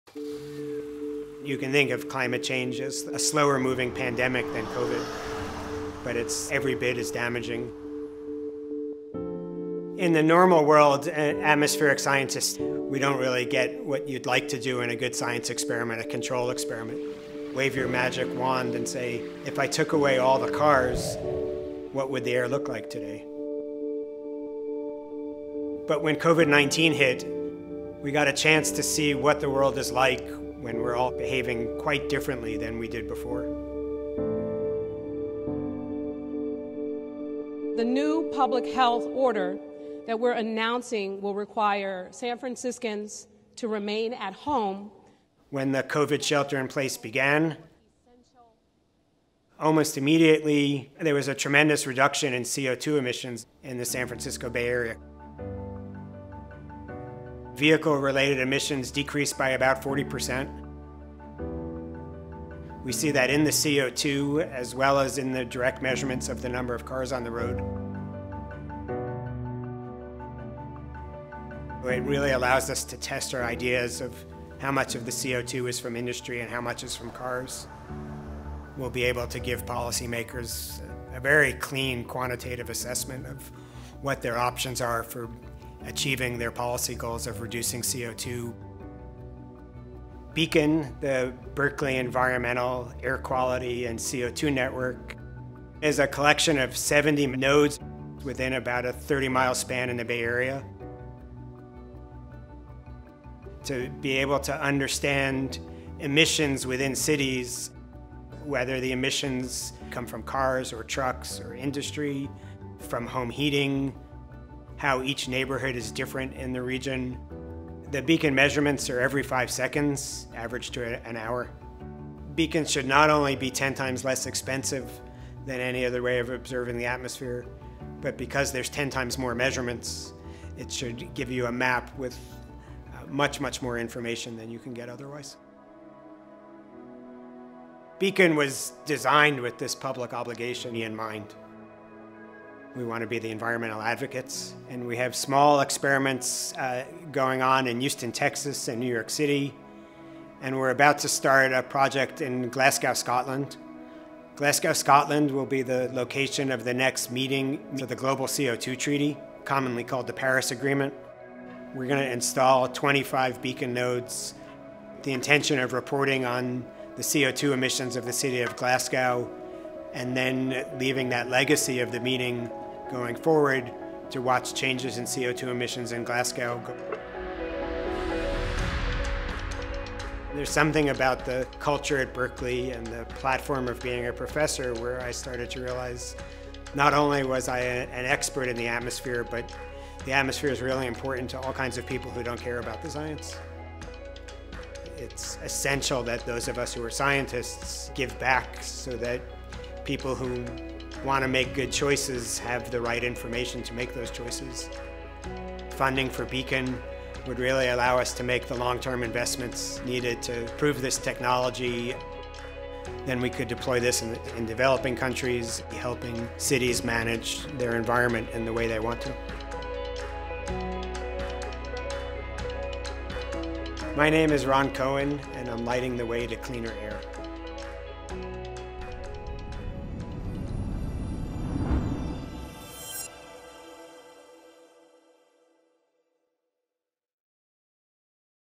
What did COVID-19 do to the Bay Area’s air quality?
When the spread of COVID-19's forced a shelter-in-place, Professor Ron Cohen knew it was the perfect opportunity to test changes in air quality in the San Francisco Bay Area using BEACON, UC Berkeley's novel air-quality and CO2 network.